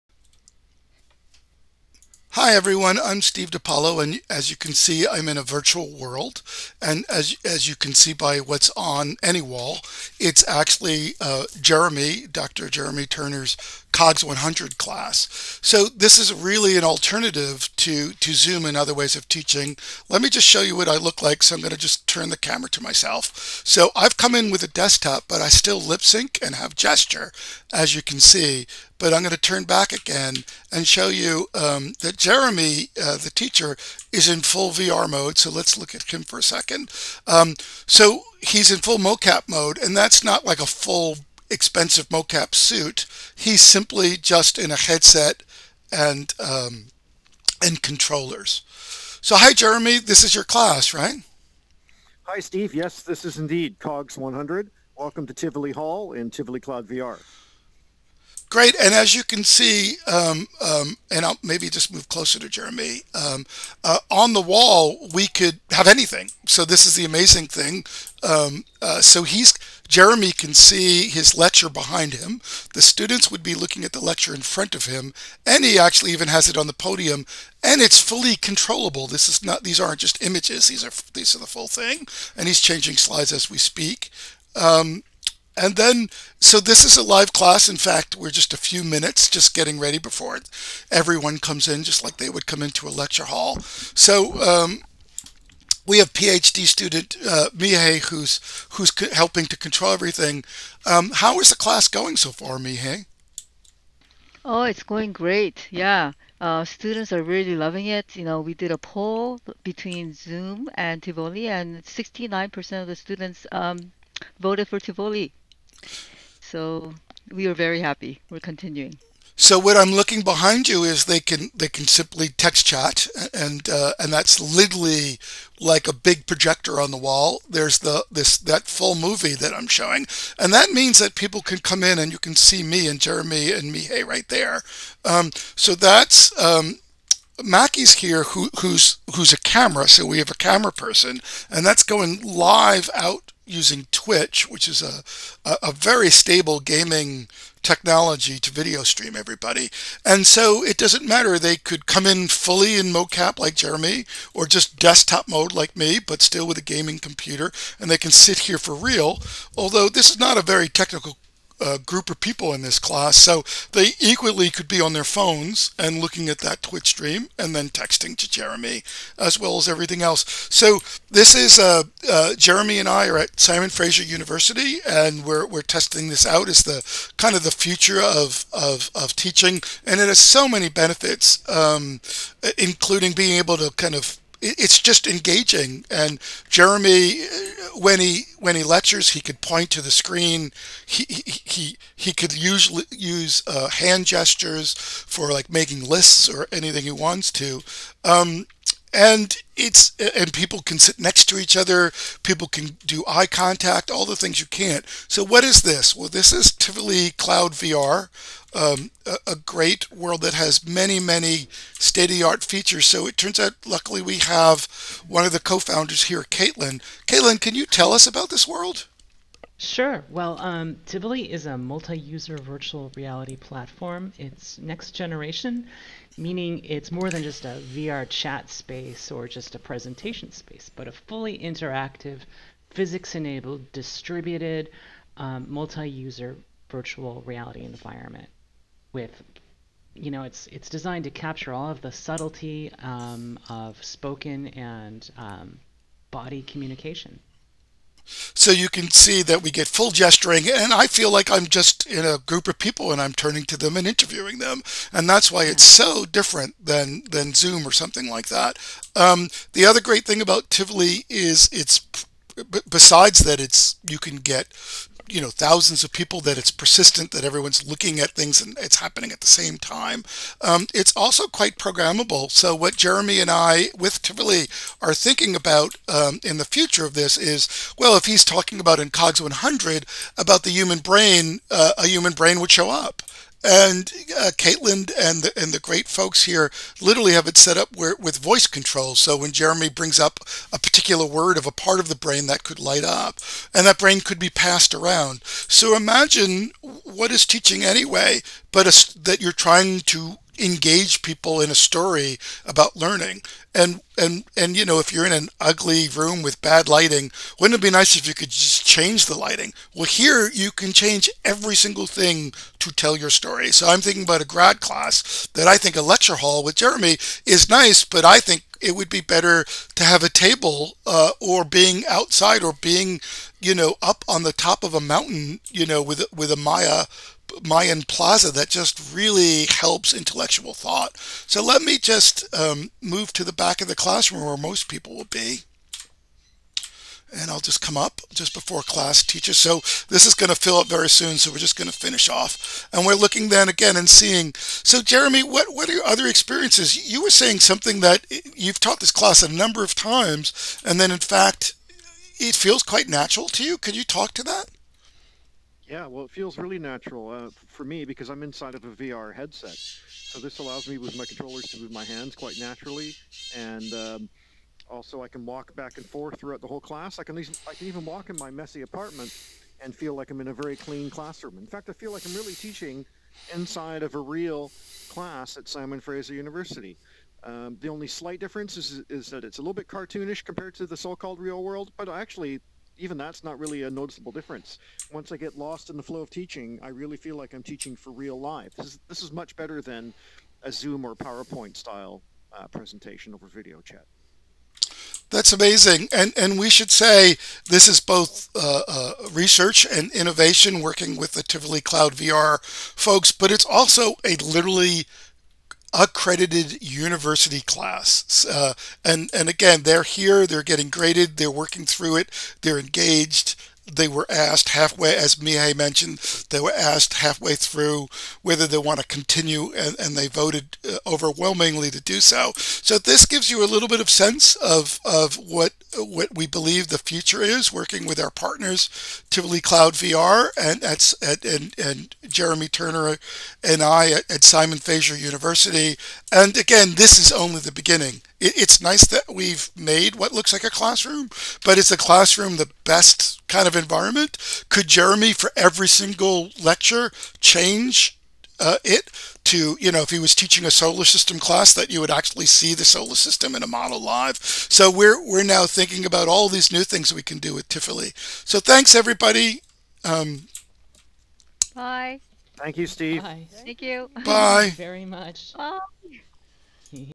Teaching in Full VR for an SFU university class
Just before this accredited Univ class at SFU begins its online lecture  (it is COGS100 - intro to cognitive science), I virtually interviewed the teacher/researcher (in full VR with natural movement)  and other researchers on the future of immersive remote teaching.